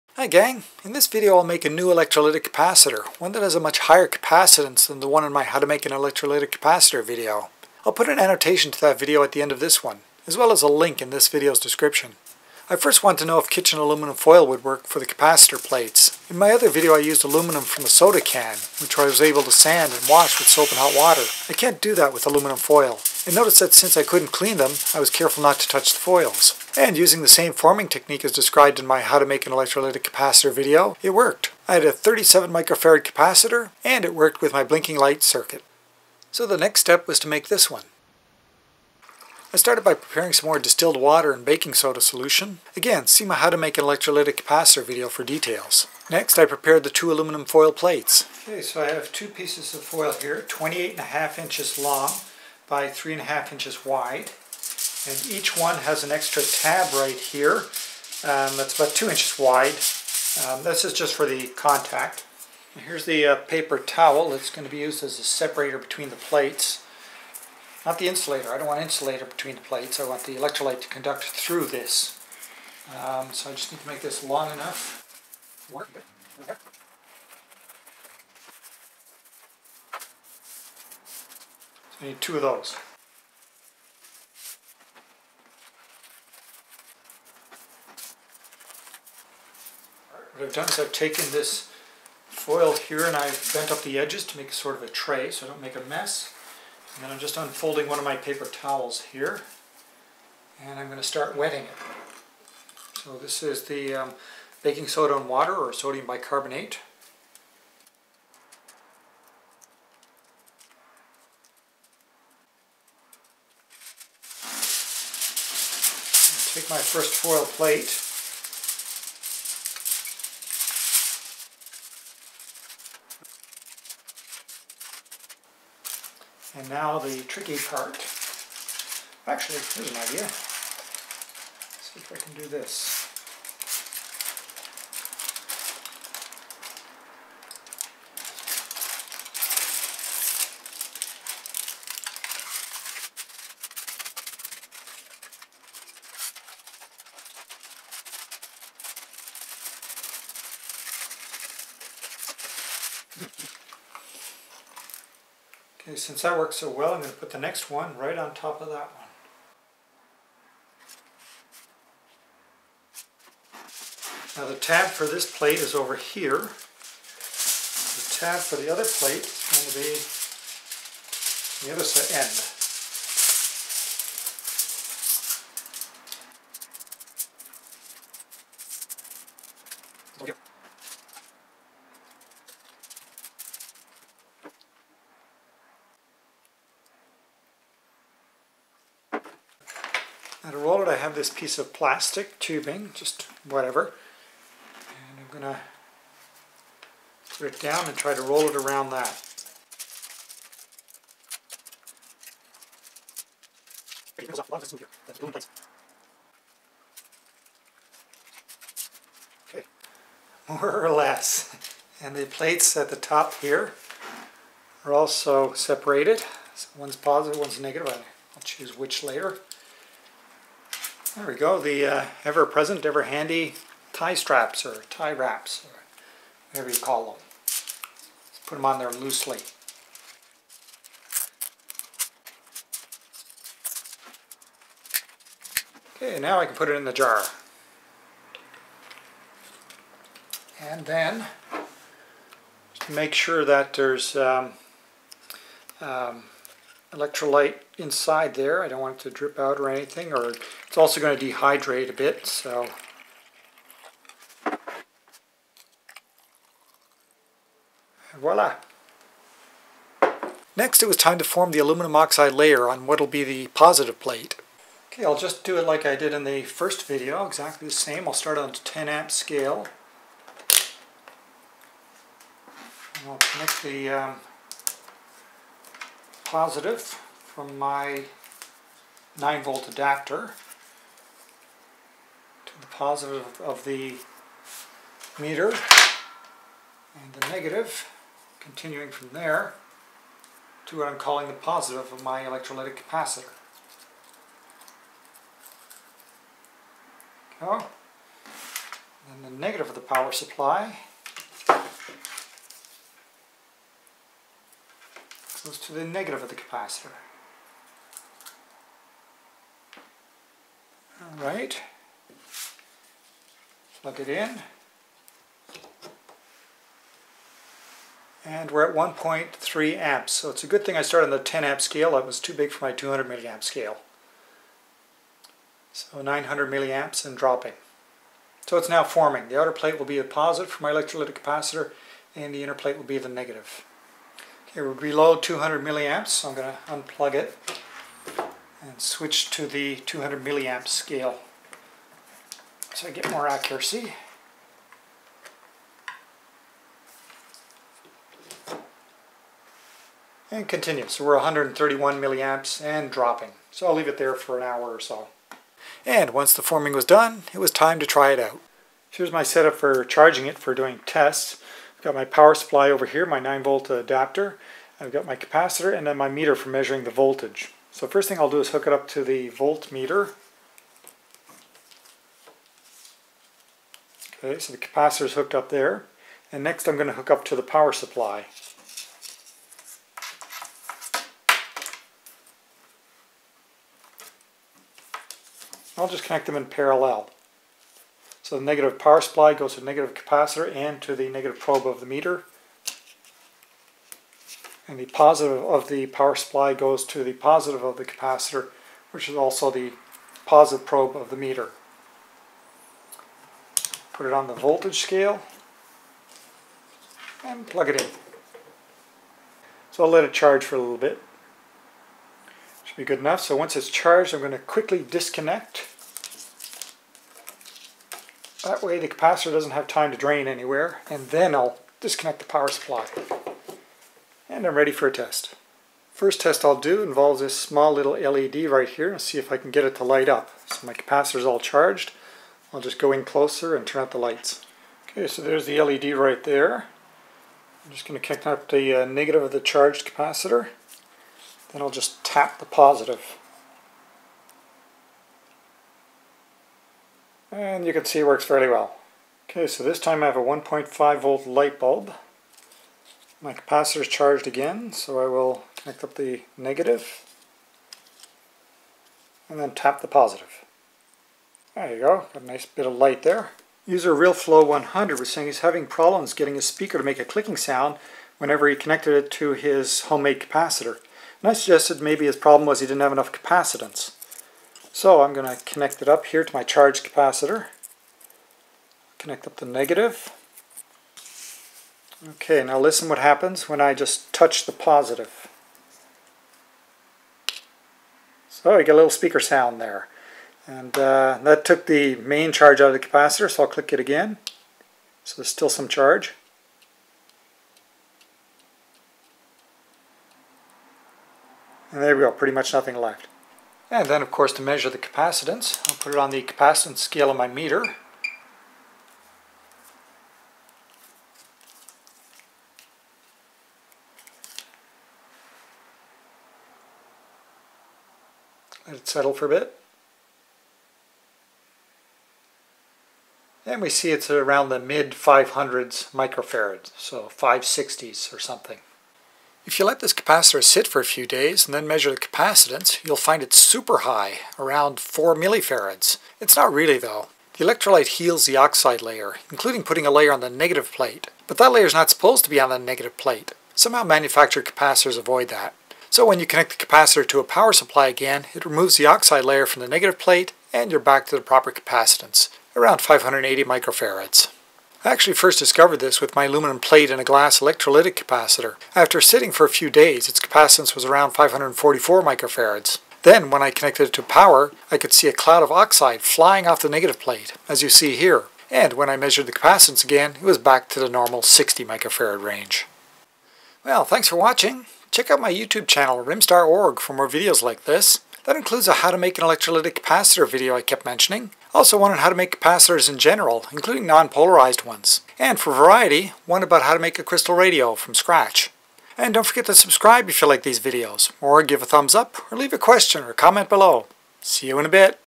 How to Make a High Capacitance Electrolytic Capacitor
Step-by-step demonstration of how to make high a capacitance electrolytic capacitors from home with household parts - aluminum foil, paper towel, distilled water and baking soda (sodium bicarbonate.) The capacitance of the one made here is around 560 microfarads.

For the detailed basics and explanations on making homemade electrolytic capacitors, see my "How to Make an Electrolytic Capacitor" video: